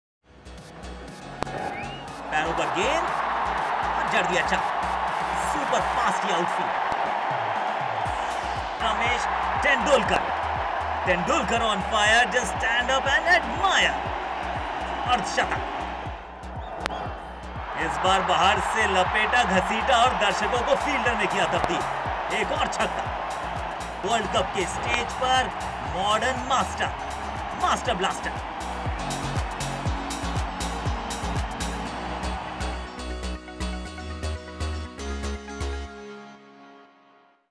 Sachin Tendulkar | Wonderfull Century | India vs England | 2011 World Cup
Sachin Tendulkar made wonderfull century in ICC mens cricket world cup agains England.
.Enjoy the highlight in hindi commentary.
Jai Hind.